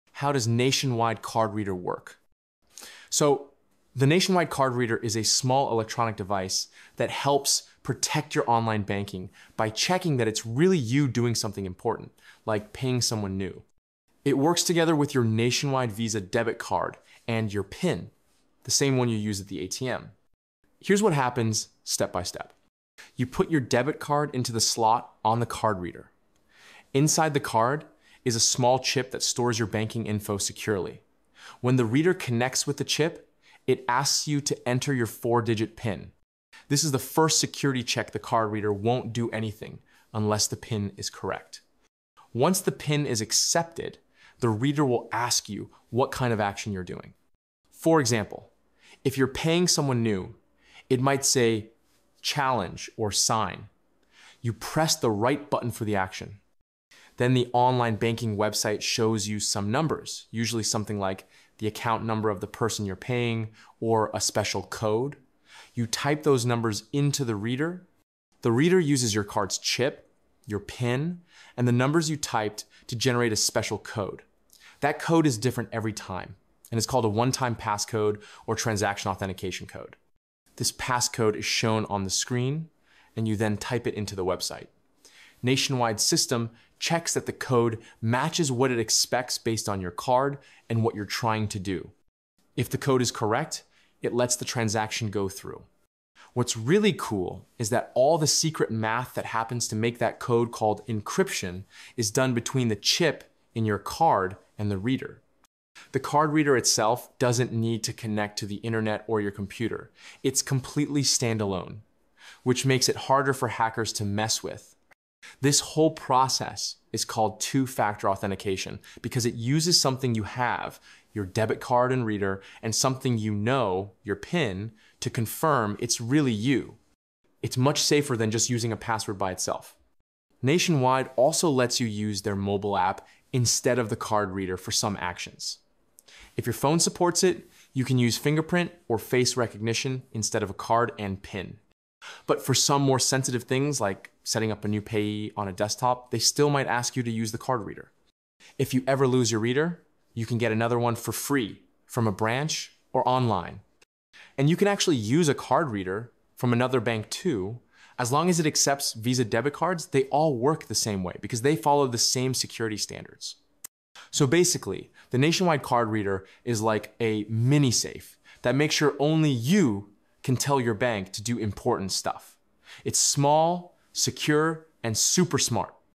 How Does Nationwide Card Reader Work 2025: Explained!
Learn How Does Nationwide Card Reader Work. here is how to use nationwide card reader : The Nationwide card reader works by generating a unique code when you insert your debit card and enter your PIN. This code is used to securely authorize online banking transactions like setting up new payees or making payments.

How to Tech is the home of technology-based how-to's, guides & tips.

How To Tech